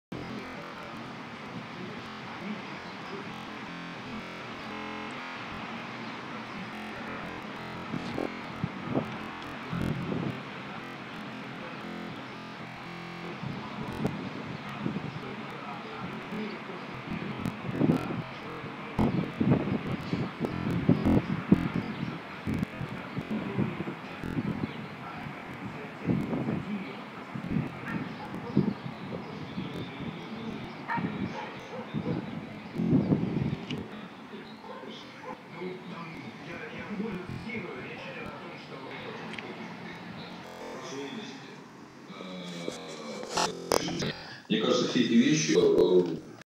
Yekaterinburg Storm is coming Екатеринбург Гроза наступает
Хроника переменчивой погоды над нашим городом:
Центр в солнечном ореоле, горизонт в дожде, Запад покрыт тьмой, на Юге и Востоке - все безоблачно